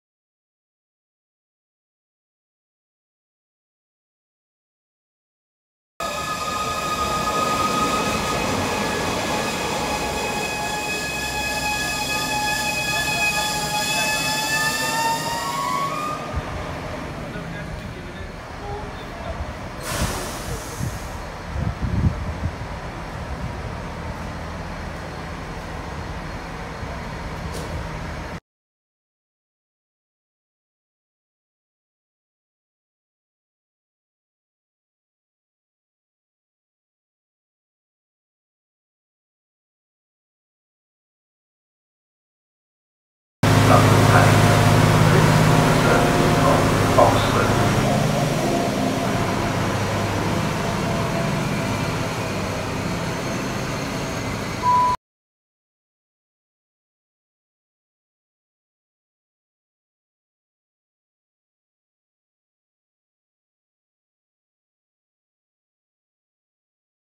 Unimat Tampers At Slough Station
This wonderful piece of machinery stopped for enough time to create my video